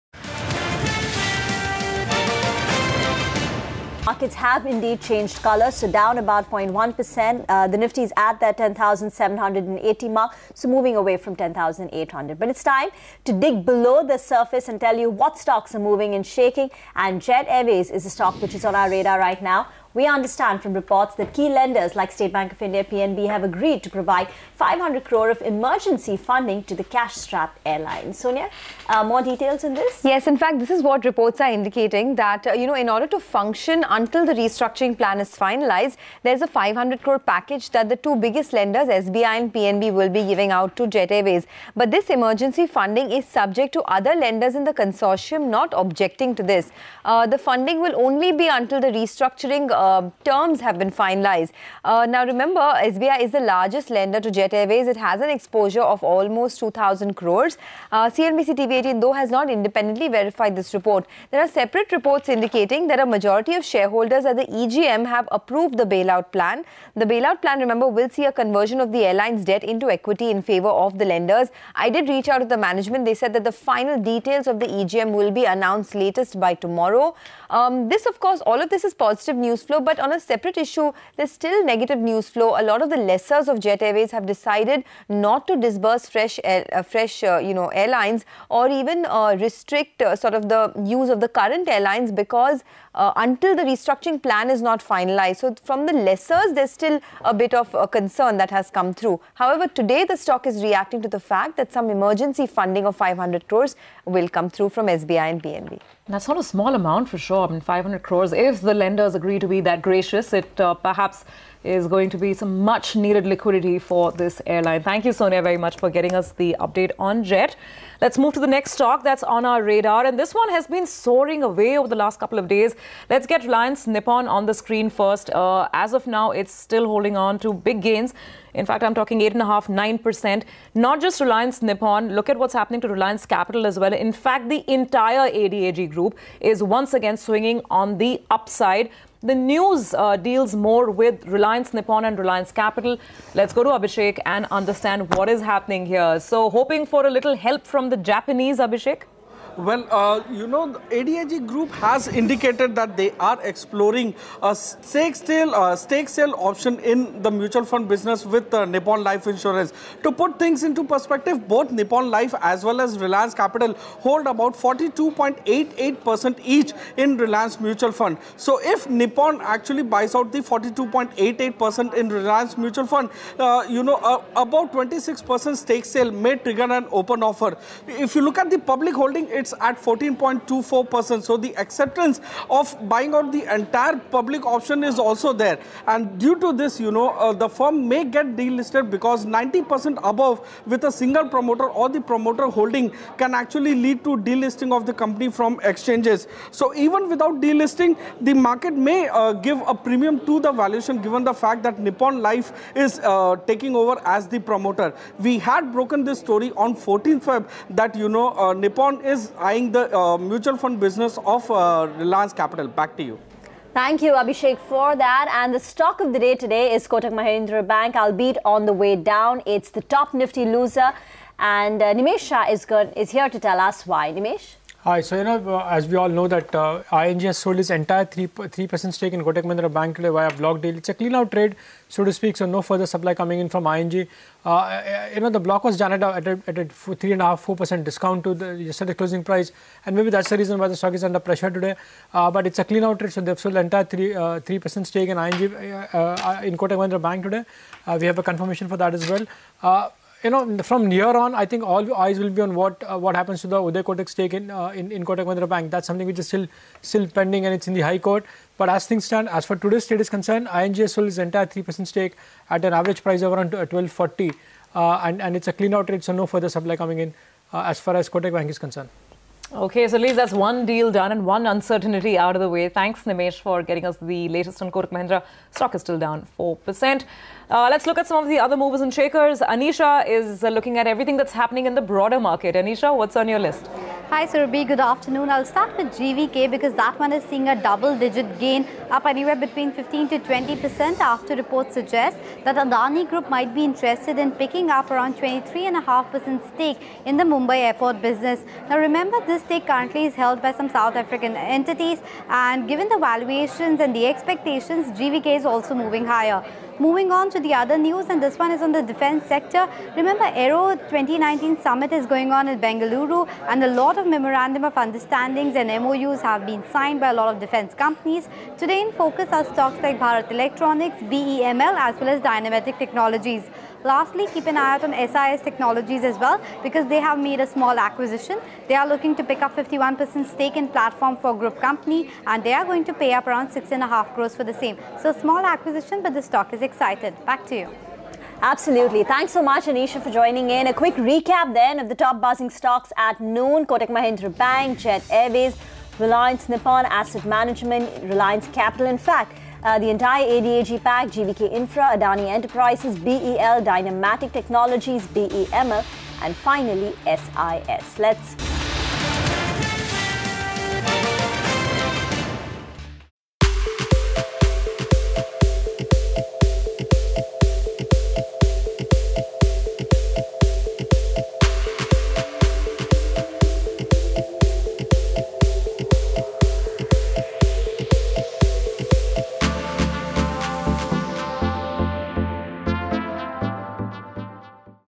Jet Airways' Shareholders Question Resolution Plan, Says Report
Jet Airways' minority shareholders questioned the airline's resolution plan as well as the board's lack of transparency after the company held an extraordinary general meeting with them on Thursday to discuss the bailout plan.